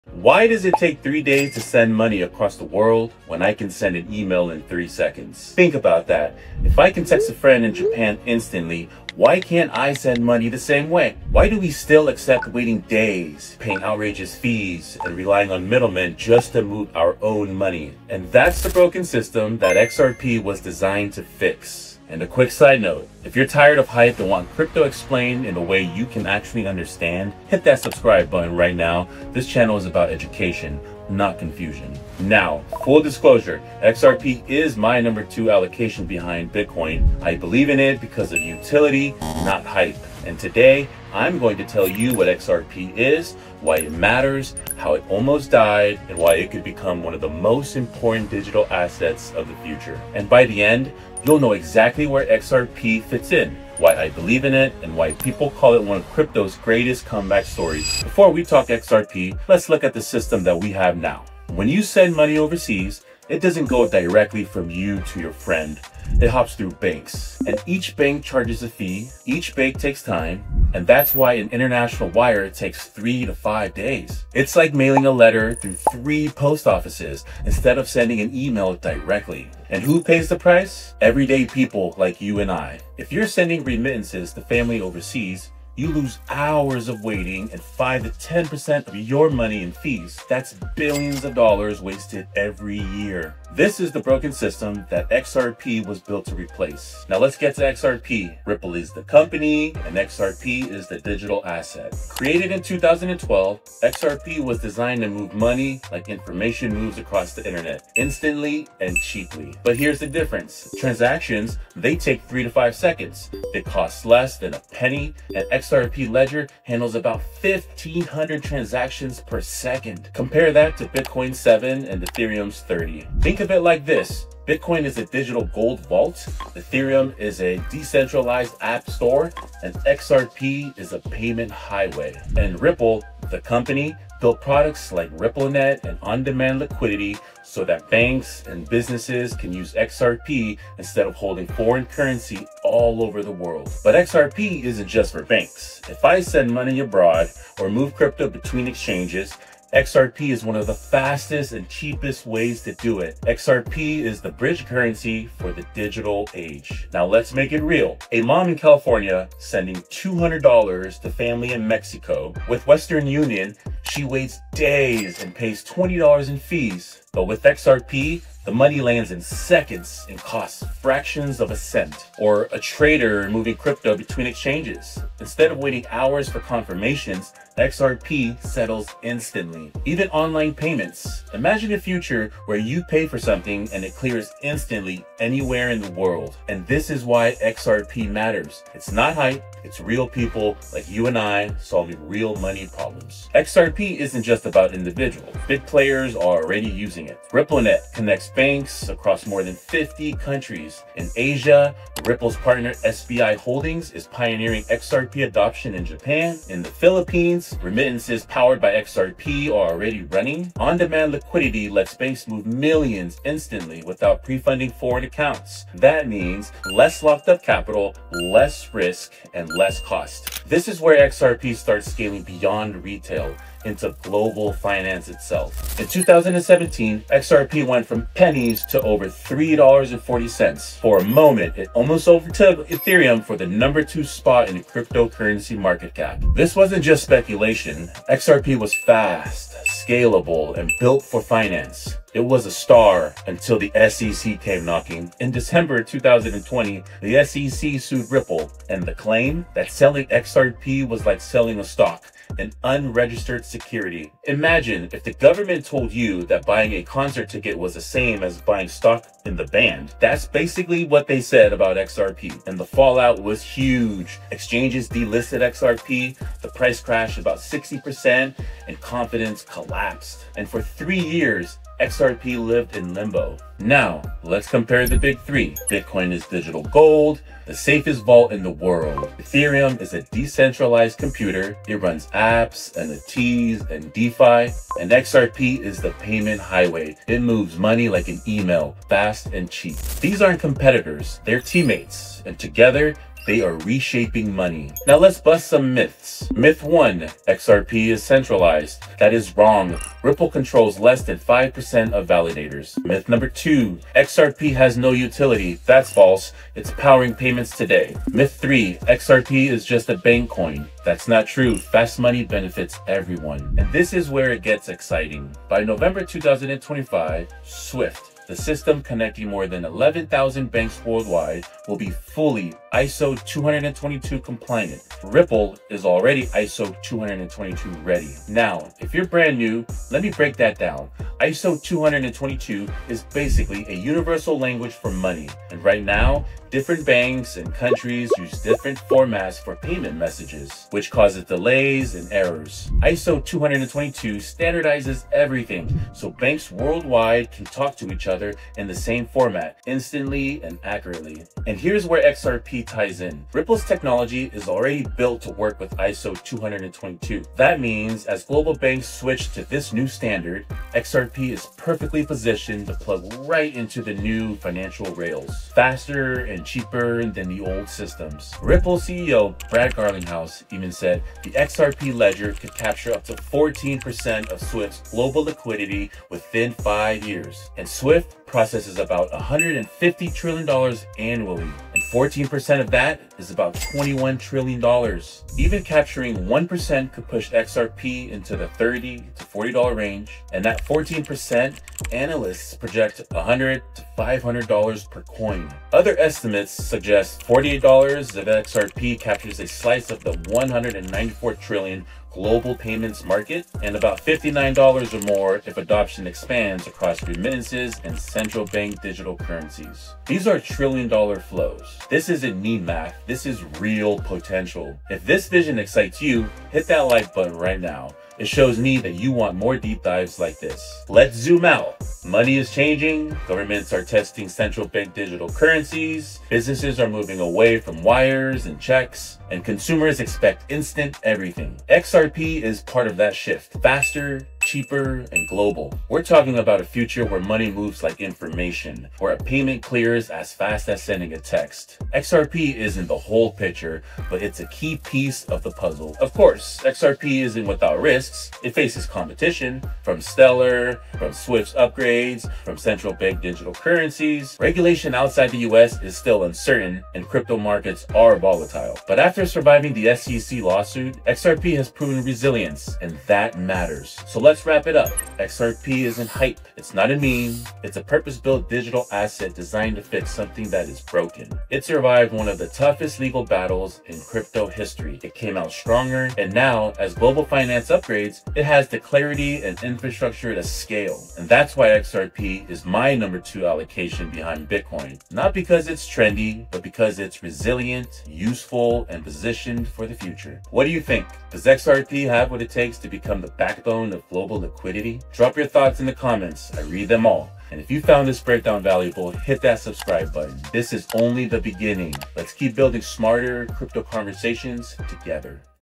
Why Sending Money Is Still Broken — And What XRP Was Built to Do
Why does sending money across the world still take days in a digital-first era? In this video, I break down why global payments are slow and expensive — and how XRP was designed to improve the system.

This is a beginner-friendly XRP explanation, focused on real-world use cases, payment infrastructure, and how modern financial systems are evolving.

If crypto has ever felt confusing, this deep dive is built to give you clarity without hype.

In this long-form breakdown, you’ll learn:
- Why international payments rely on outdated systems
- How XRP is designed to move value quickly and at low cost
- The difference between Ripple, XRP, and the XRP Ledger
- Real-world examples of XRP use cases and remittances
- A simple overview of the SEC case and what changed
- What ISO 20022 is and why global financial systems are upgrading
- How digital assets fit into the future of global payments

This is my deep dive into Ripple's XRP, created to help everyday people understand XRP’s role in modern finance — without jargon, hype, or speculation.

I read the comments — what do you think about XRP’s real-world utility?

Key concepts covered:
- Cross-border payments and settlement speed
- Transaction cost and scalability
- RippleNet and global payment connectivity
- ISO 20022 explained simply
- Blockchain technology in financial infrastructure

📩 Join the Smash Crypto Zone Newsletter
Get my best crypto insights — direct to your inbox.